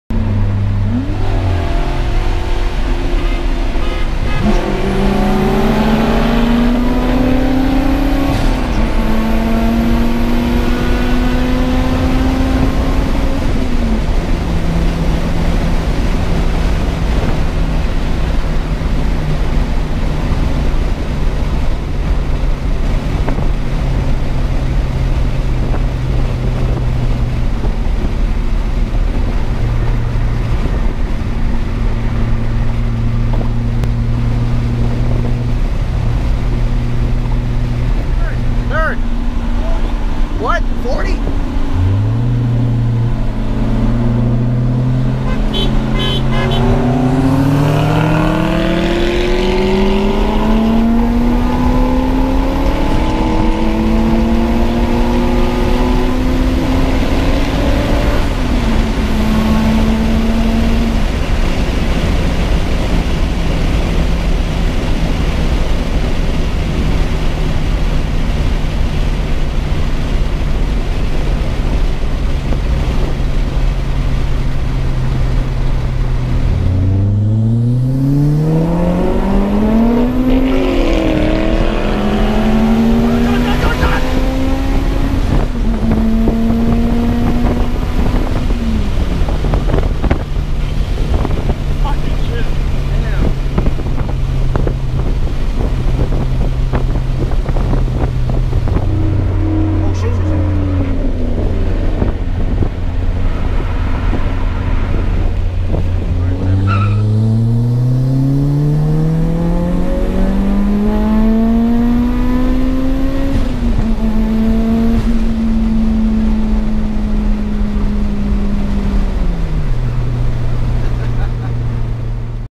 Turbo Celica GTS Vs. Mustang GT 2010
Newest upload: "S14 Growing Pains... + Kouki Front End!!!"

The turbo Celica is holding up a good battle with the Mustang! Definitely worth all the time installing. Mustang has exhaust system done to it while celica has turbo charger running 6.5-7.5 lbs of boost (boost gauge is off by a bit).

Interested in making some quick money just by searching on the internet? try out qmee today! It's 100% free!

Hope you guys enjoyed the video! leave a rating please! and have a nice day.